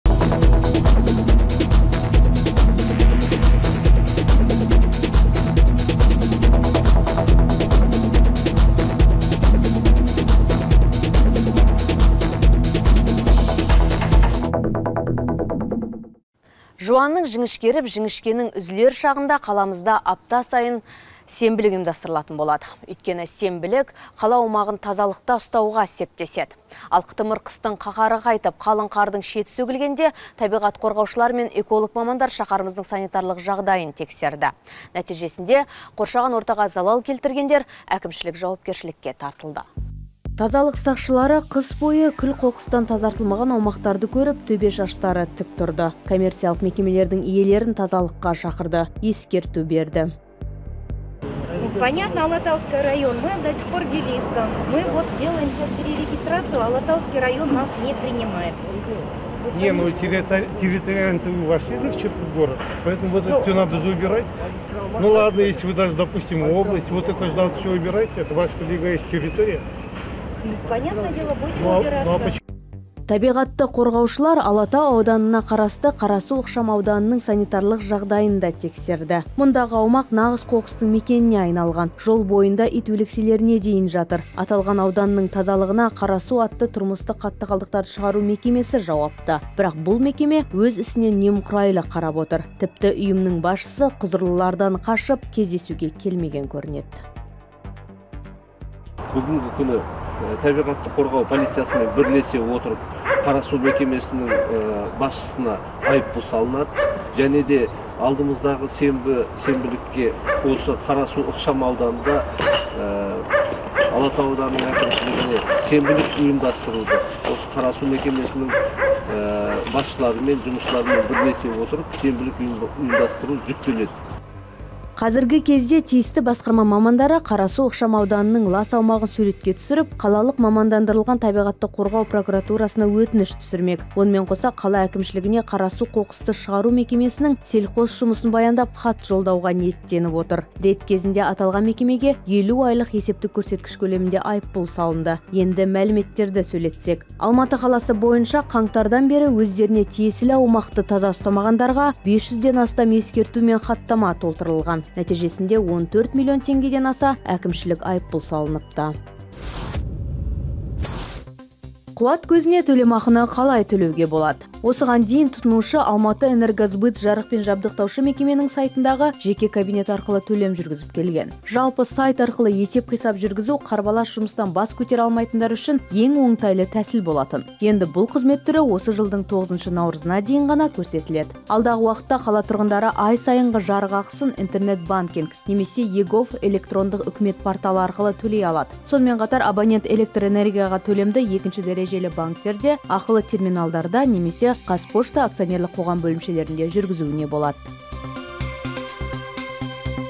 Алматыда апта сайын сенбілік өтеді (01.03.16)
Өйткені сенбілік қала аумағын тазалықта   ұстауға  септеседі. Ал қытымыр қыстың қаһары қайтып,  қалың қардың шеті сөгілгенде  табиғатты қорғаушылар мен эколог мамандар шаһарымыздың санитарлық жағдайына тексеру жұмыстарын бастап кетті.   Нәтижесінде қоршаған ортаға залал келтіргендер әкімшілік жауапкершілікке тартылды.